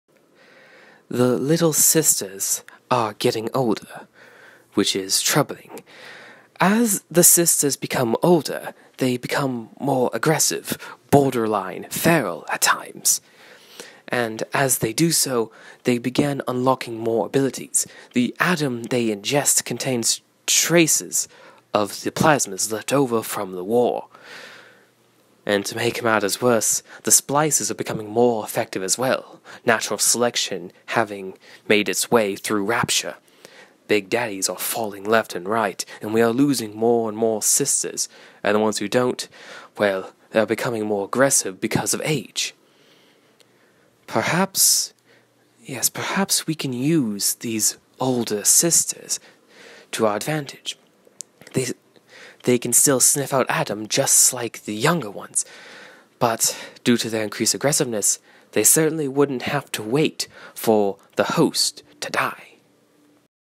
BioShock Impression-Gilbert Alexander
A vocal impersonation of the timid scientist from BioShock 2.

I do not BioShock 2 or any of its characters.